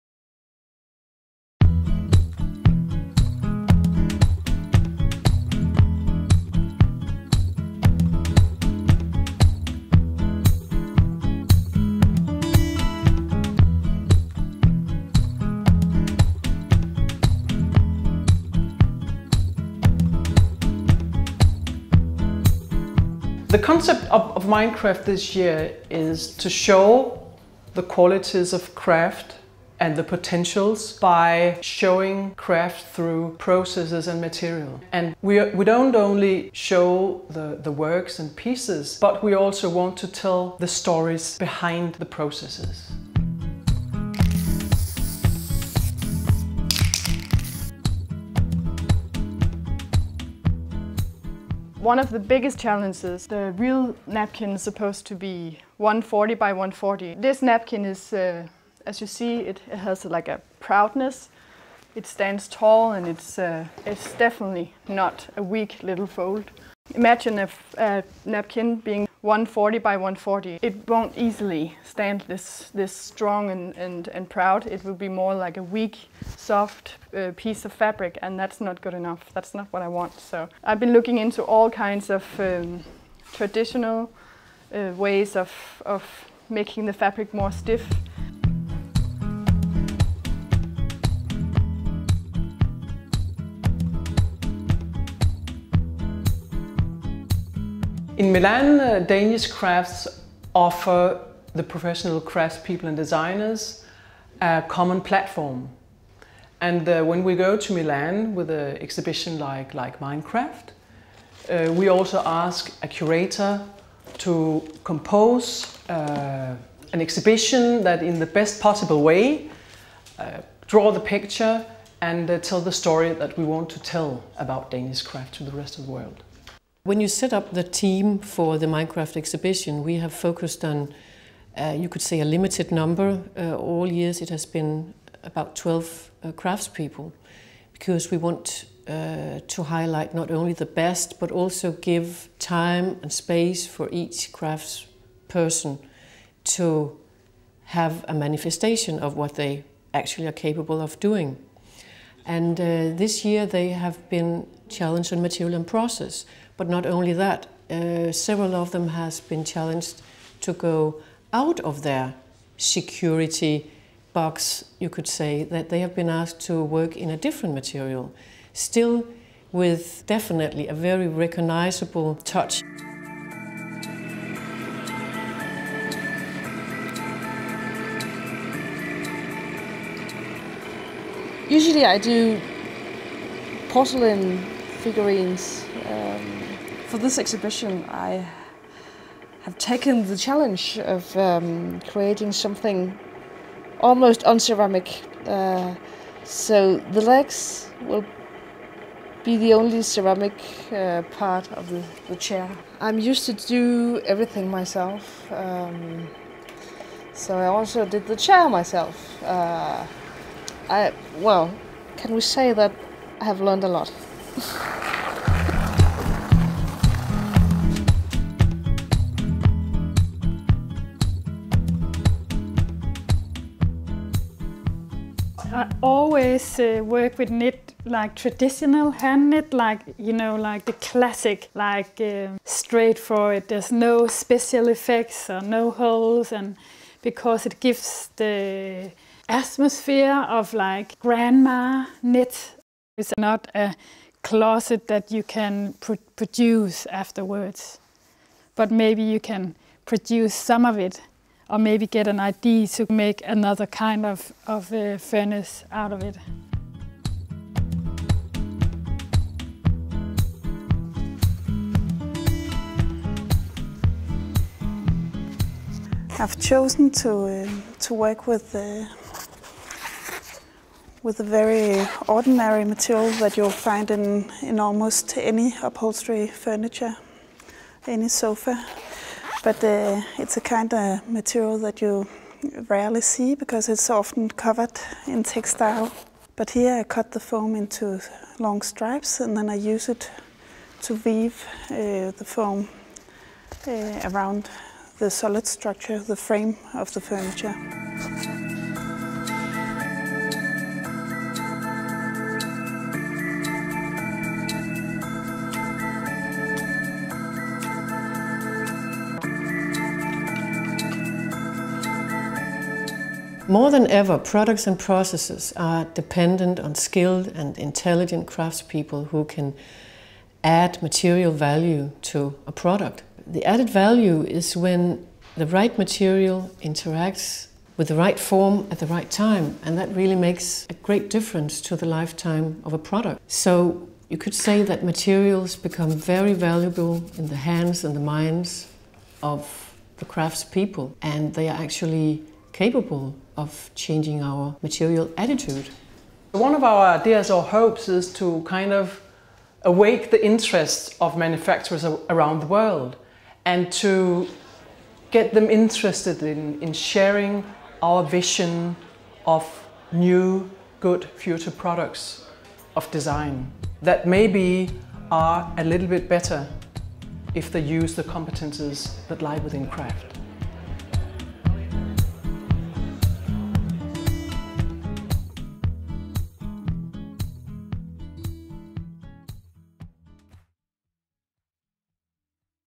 Danish Crafts - MINDCRAFT 10
DANISH CRAFTS presented MINDCRAFT10 in Zona Tortona during Milan Design Week 2010. An exhibition featuring 12 brand-new works from a selection of Denmarks finest designers and craftspeople.